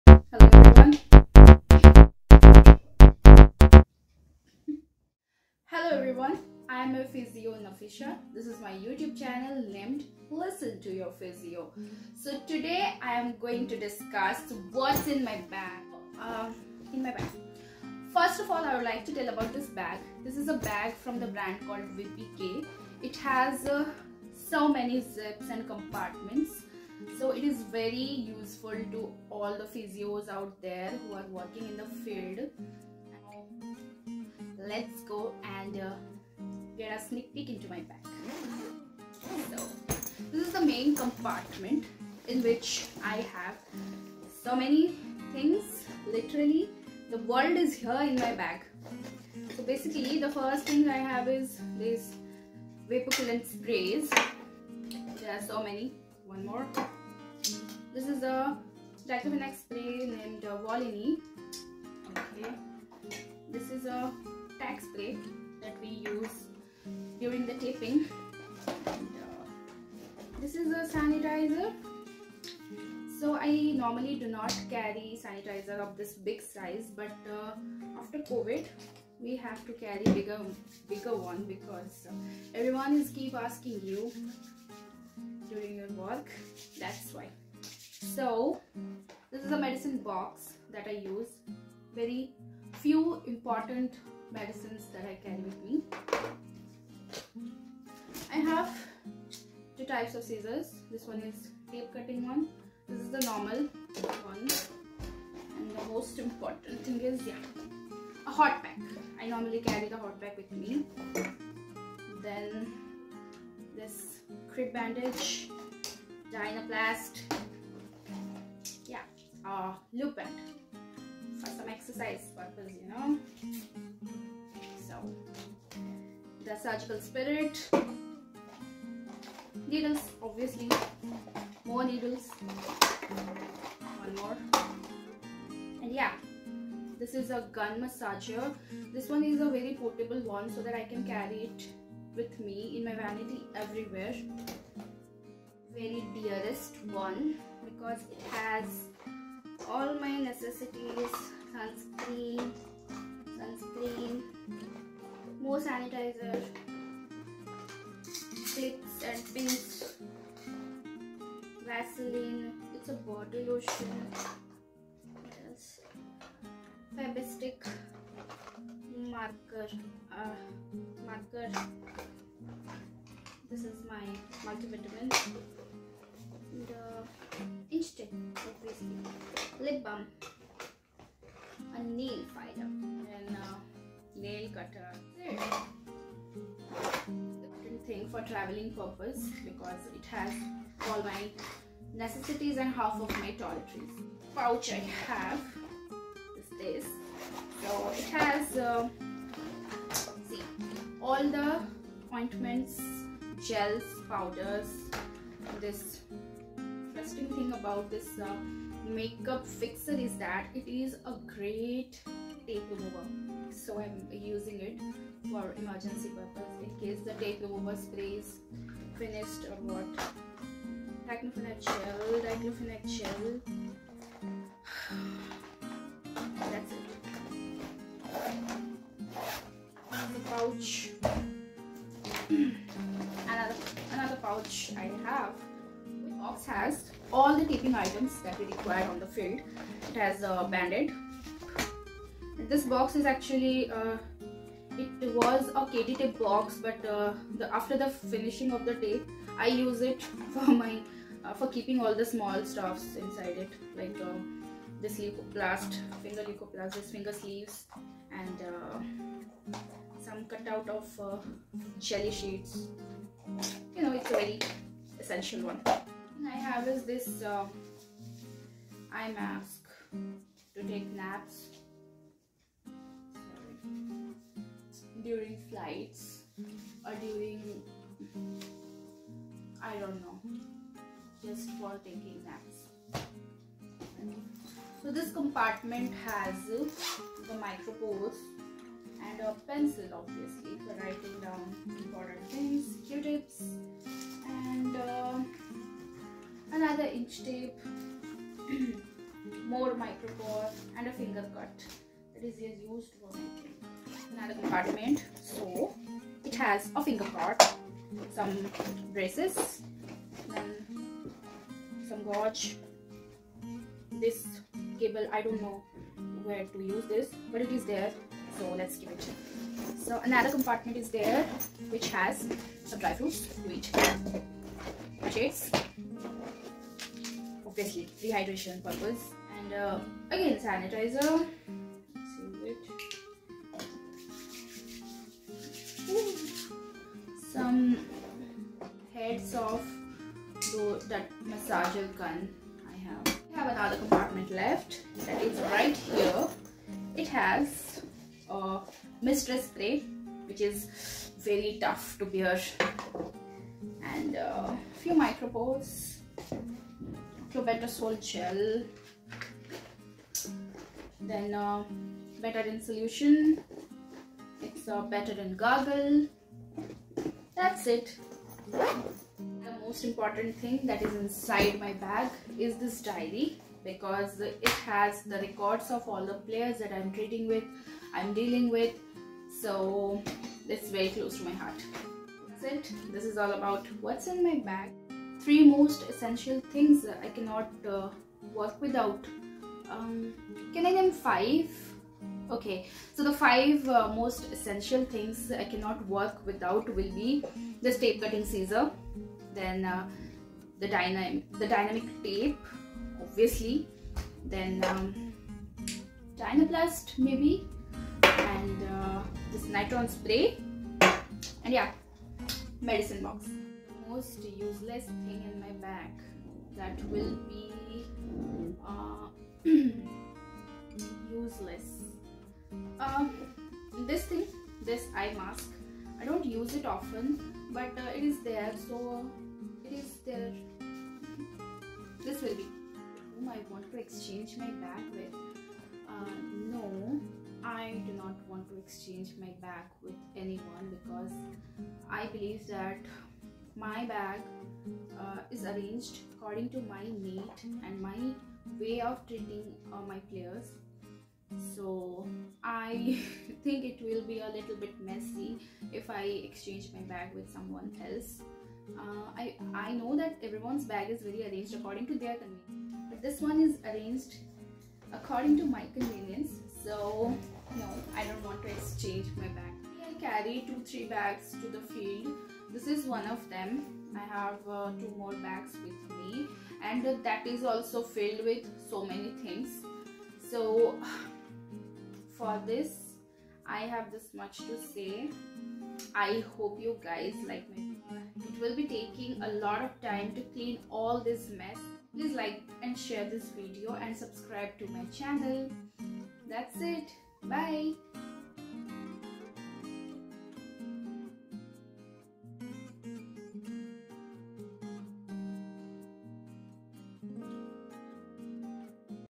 what's in my bag?/Episode-1
so,as a sports physio,we carry so many things in our bag.In this video I have shown some of the things in my physio bag.Definitely,not all the things.